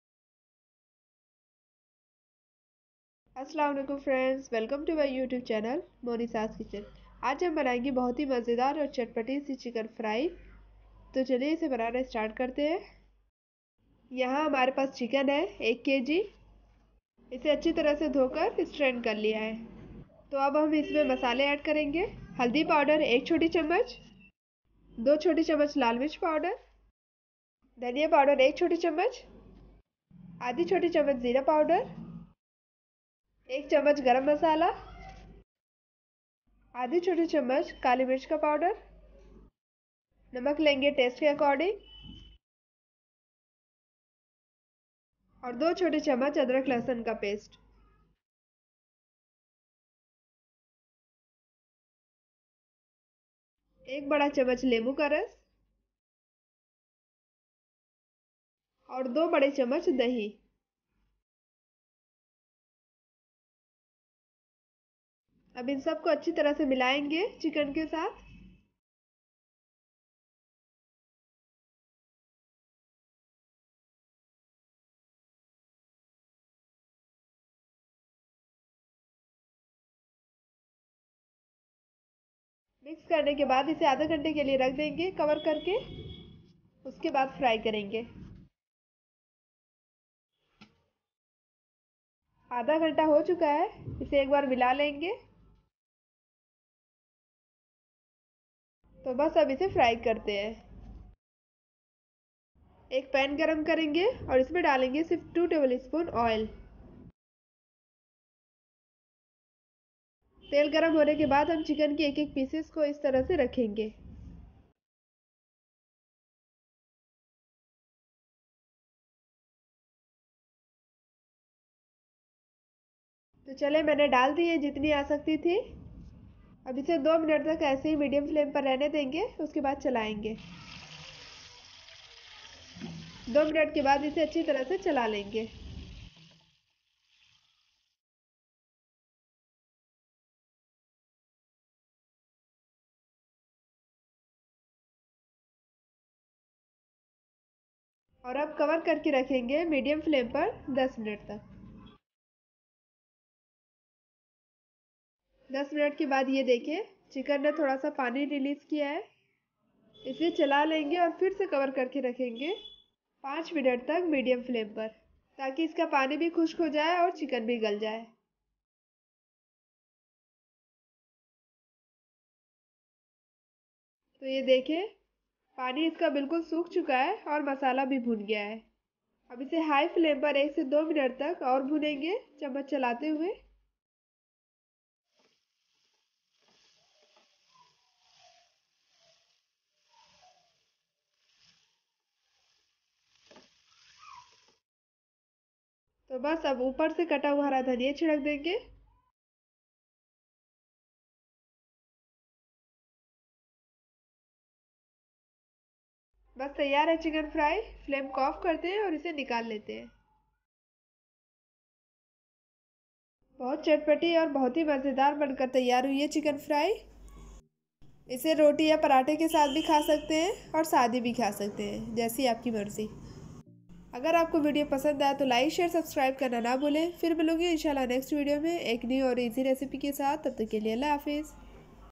Chatpata Chicken Fry | Chicken Fry in Only 2-Spoon Oil By Monisa's kitchen
Chutpata Chicken Fry | Chicken Fry in Only 2-Spoon Oil By Monisa's kitchen

1.Chicken fry
2. Chicken fry recipe
3.fried  chicken
4.chupata chicken fry
5.Oil free recipe

I hope apko meri recipe pasand ayi hogi...

Don't forget